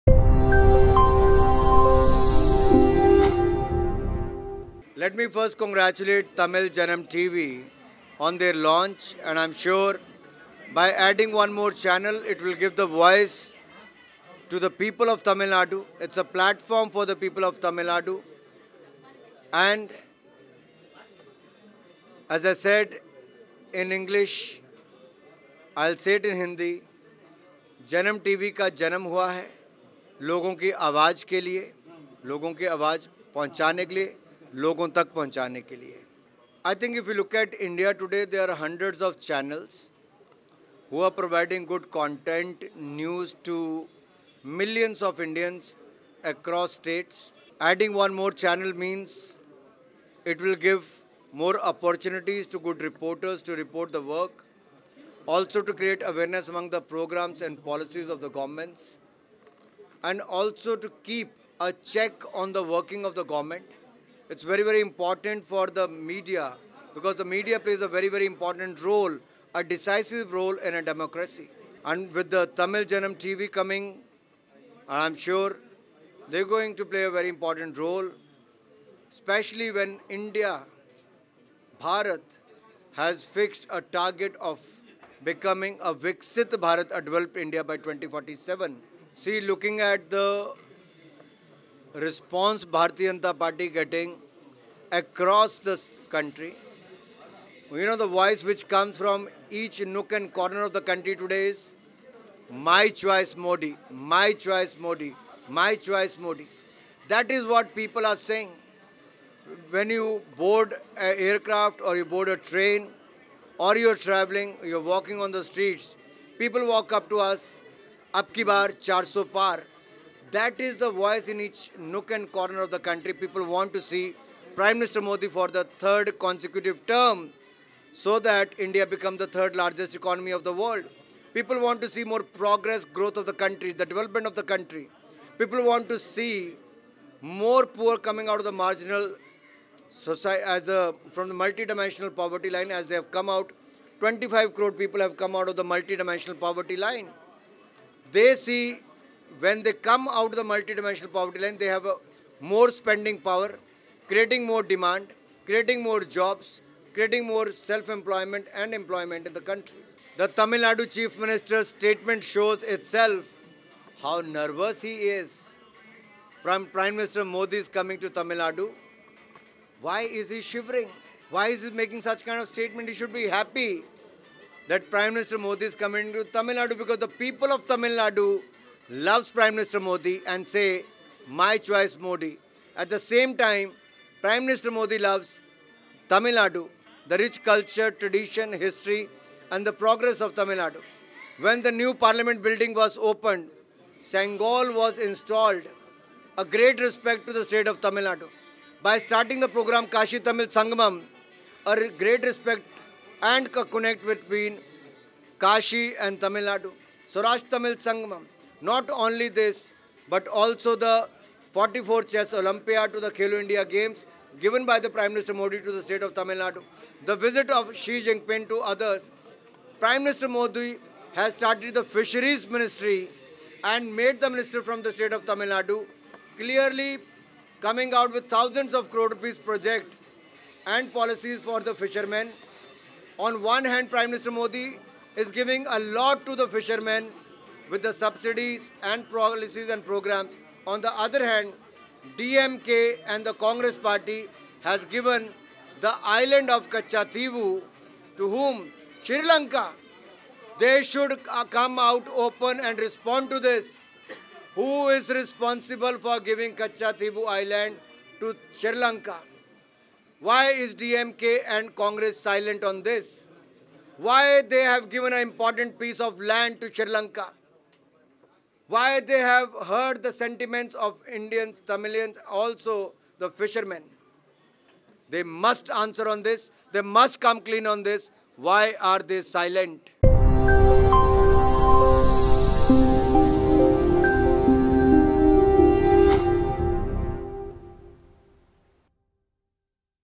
அனுராக் தாக்கூர் செய்தியாளர் சந்திப்பு - Anurag Thakur Latest Pressmeet at Tamil Janam Launch Event
Anurag Thakur Latest Pressmeet at Tamil Janam Launch Event .